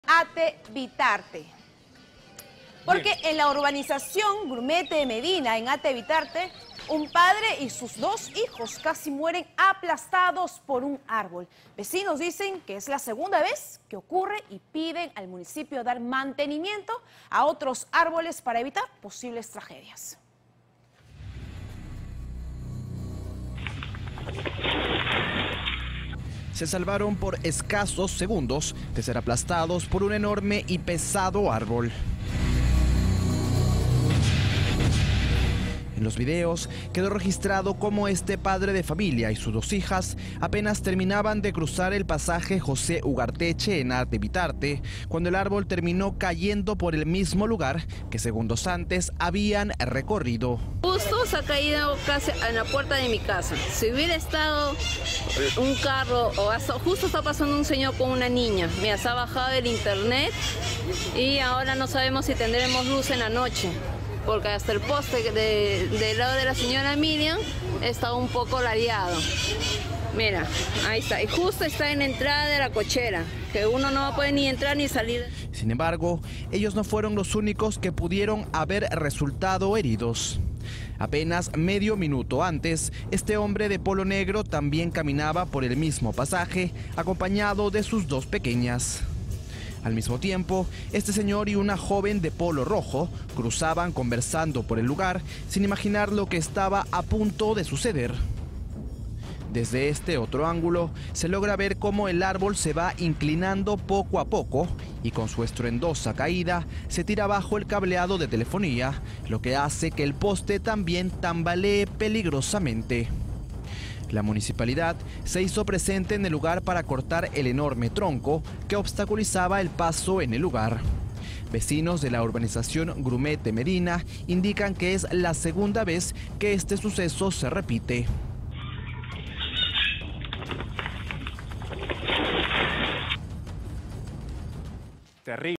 Árbol cae y casi mata a padre y sus dos hijos en Ate
En el distrito de Ate, un padre de familia y sus dos pequeños hijos se salvaron de ser aplastados por un enorme árbol. En los videos quedó registrado como el progenitor y sus hijos apenas terminaban de cruzar el pasaje José Ugarteche cuando el árbol terminó cayendo por el mismo lugar que segundos ante habían recorrido.

Sin embargo, ellos no fueron los únicos que pudieron haber resultado heridos, ya que apenas medio minuto antes otro vecino también caminaba por el mismo pasaje acompañado de dos pequeñas hijas sin imaginar lo que estaba a punto de suceder.

Ante este incidente, los vecinos de la urbanización Grumete Medina indican que es la segunda vez que este suceso se repite.

Cabe señalar que los residentes piden que el municipio pueda realizar un mantenimiento urgente de los arboles del parque Grumete antes de que alguien termine herido.